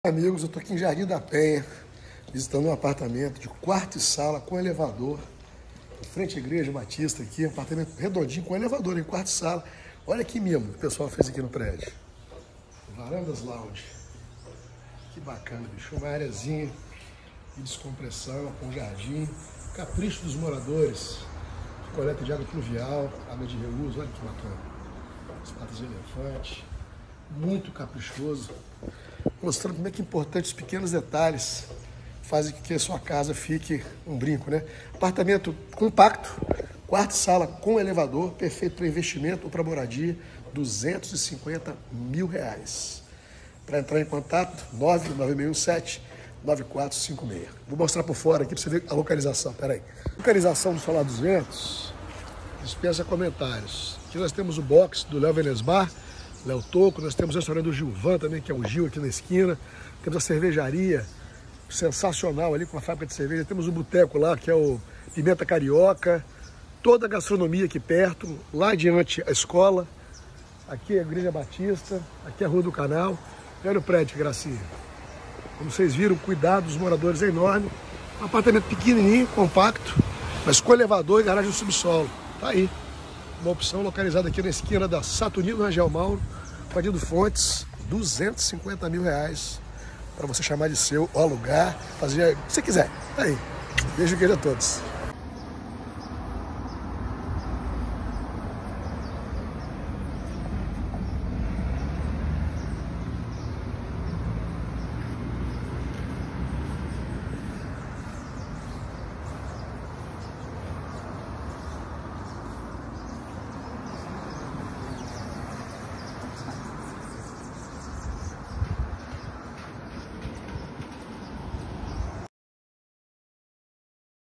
Apartamento 1 Quarto e Sala em Jardim sa Penha, venda, R$ 250 mil reais
Com elevador e garagem coberta, rua do Canal em frente a igreja Batista. Ed Solar dos Ventos. Zap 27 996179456